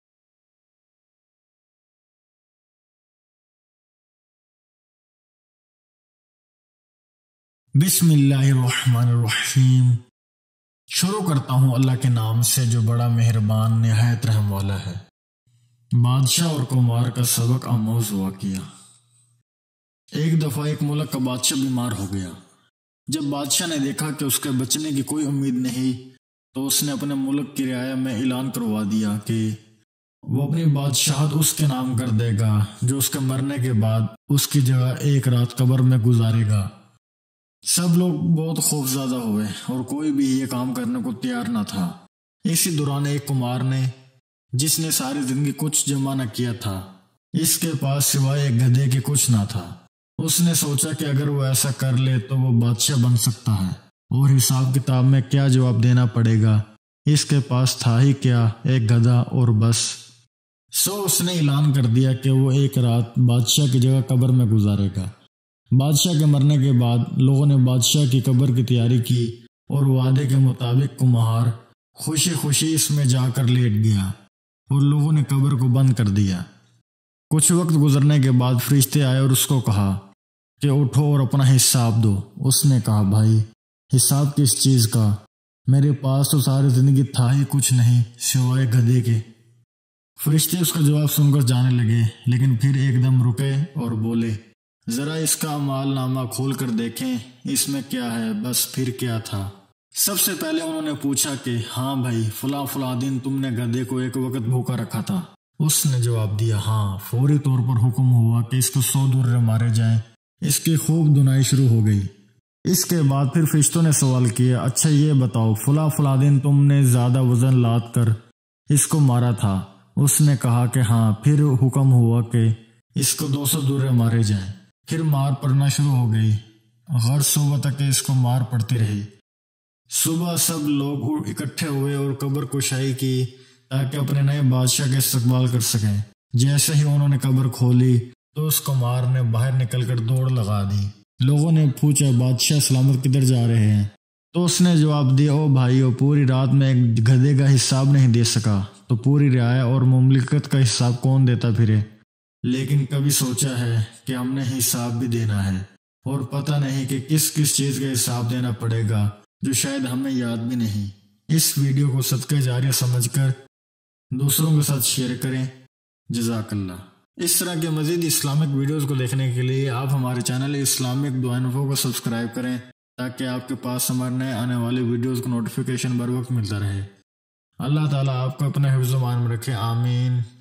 Badshah aur Kumhar ka Waqia I Islamic Story I Islamic Dua Info
In these Video Your Learn I Badshah aur Kumhar ka Waqia I Islamic Story I Islamic Dua Info
In our today's video we will tell you the instructive story of a sick king and a potter۔